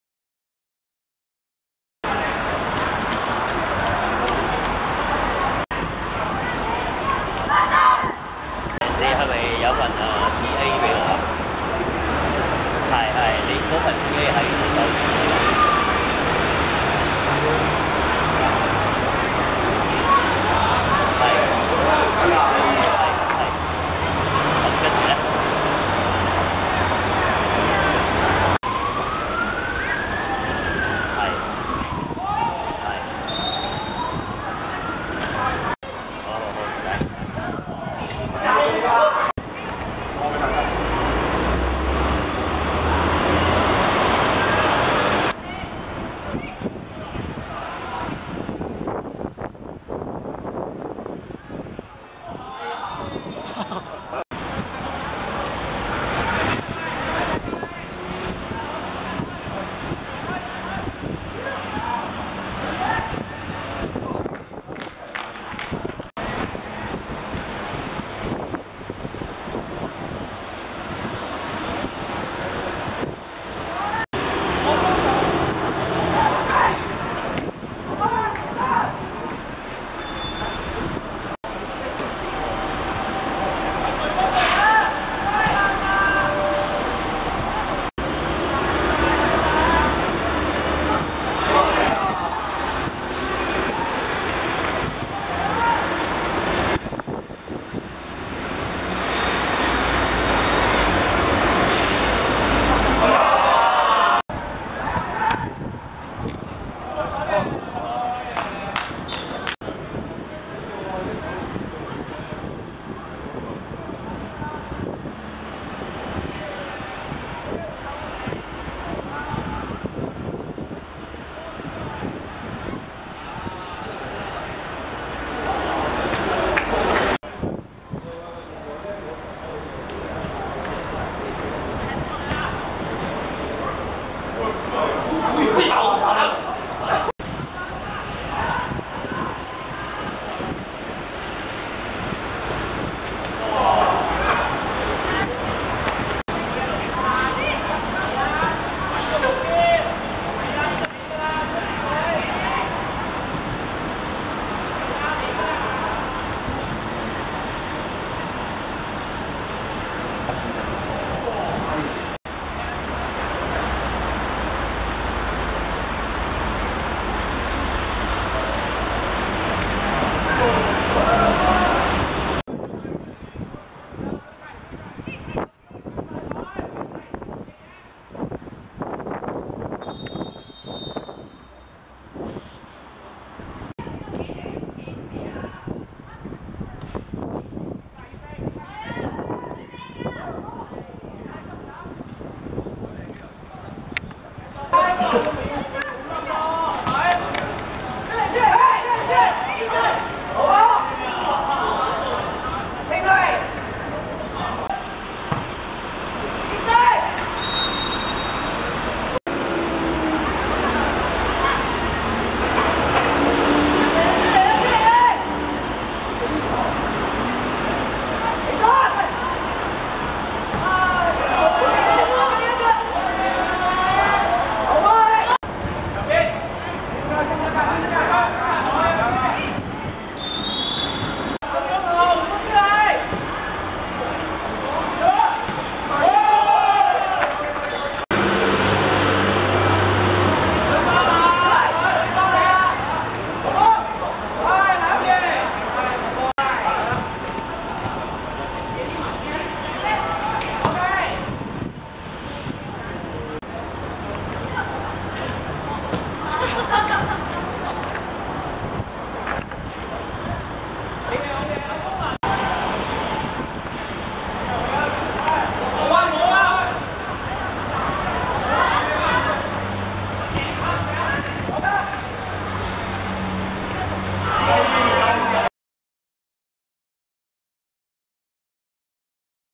拔萃vs余振強(2014.2.13.學界D1足球丙組)精華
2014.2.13.石硤尾仿真草塲 17:00 拔萃(白) 4:0 余振強(黃)